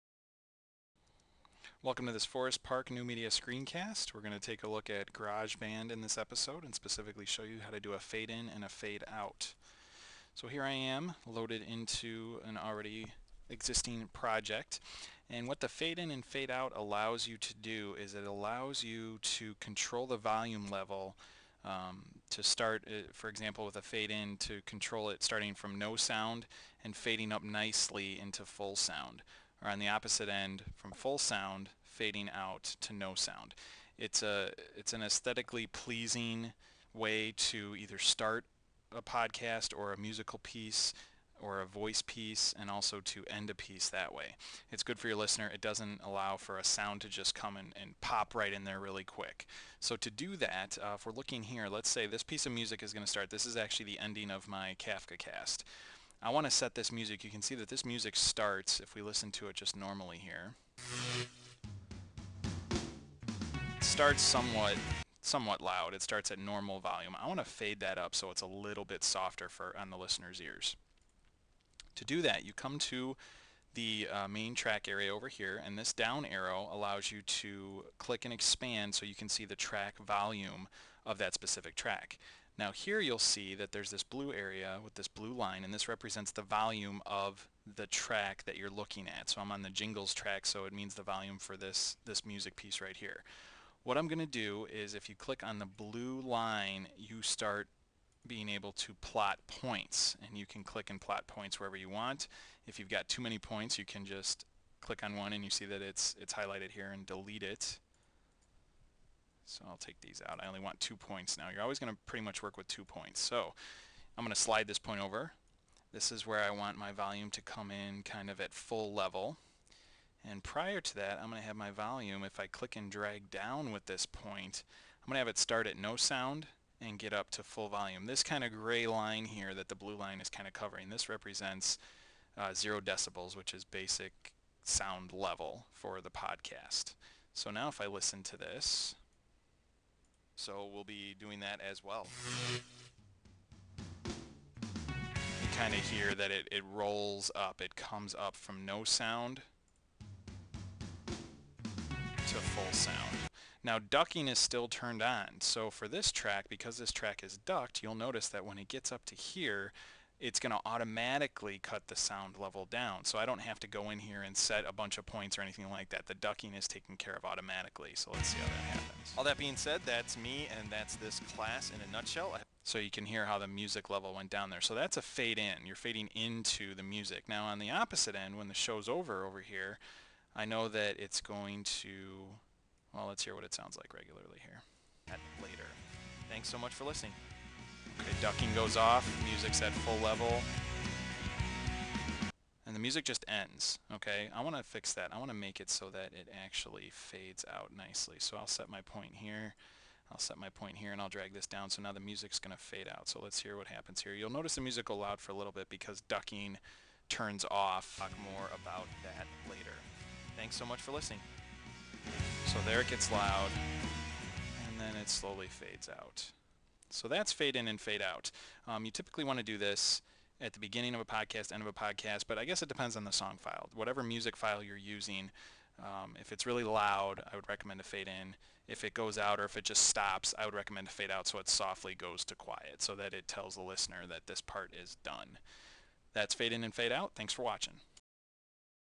6 - GarageBand: Fading In & Out
This video shows how to set volume points in GarageBand to FADE sounds IN and OUT.  This relates to GarageBand '06, '08, and '09.